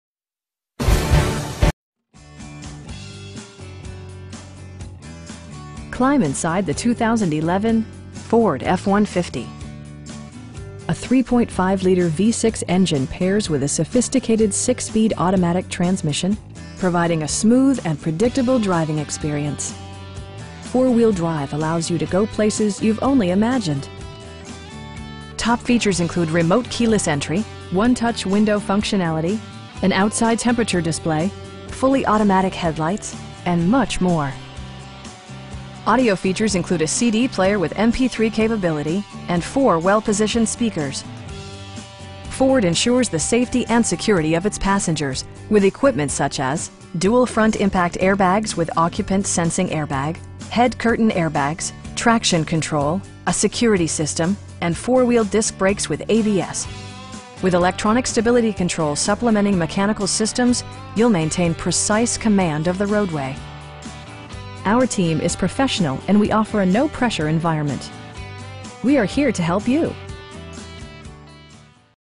2011 Ford F-150 - Grande Prairie, AB - Stock# 14LT6173A
This 2011 Ford F-150 was used for everyday travels. Well maintained following manufacturers service warranty. This truck offers 4WD, ECO Boost, SuperCrew Cab for extra legroom and much more. GREAT for all seasons.